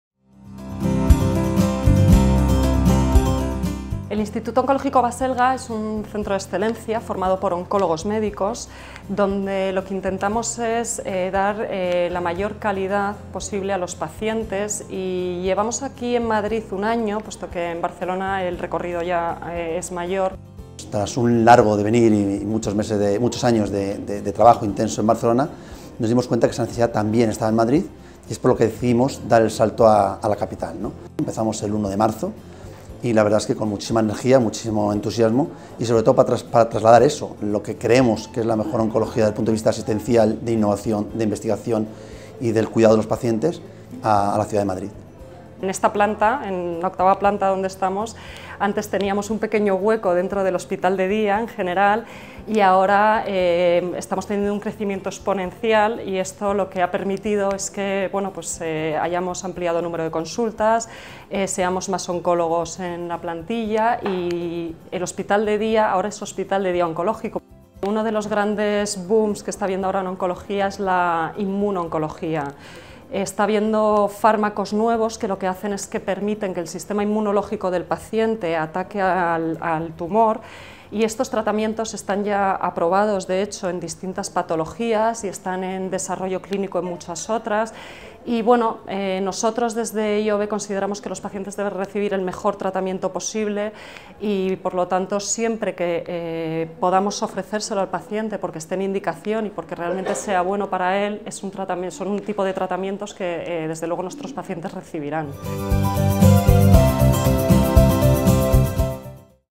El Instituto Oncológico Baselga amplía sus instalaciones de Ruber Juan Bravo
Tras un año de funcionamiento, y habida cuenta del incremento de la actividad, se ha considerado conveniente reformar las instalaciones  que ocupaba el Instituto Oncológico Baselga (IOB) en el Complejo Hospitalario Ruber Juan Bravo.
Formado por un equipo de seis especialistas en cáncer liderados por el Dr. Javier Cortés Castán: -Dra. Esther Holgado Martín, Dra. Eva Mª Guerra Alia, Dra. Marisa Torres Velasco, Dra. Teresa Alonso Gordoa y Dra. Silvia Patricia Cortez Castedo-, a los que posteriormente se han sumado el Dr. Ricardo Cubedo y el Dr. Ricardo Hitt, el IOB ocupa ahora toda la 8ª planta con un espacio de atención compuesto por 5 consultas médicas y 14 puestos de tratamiento para los pacientes oncológicos.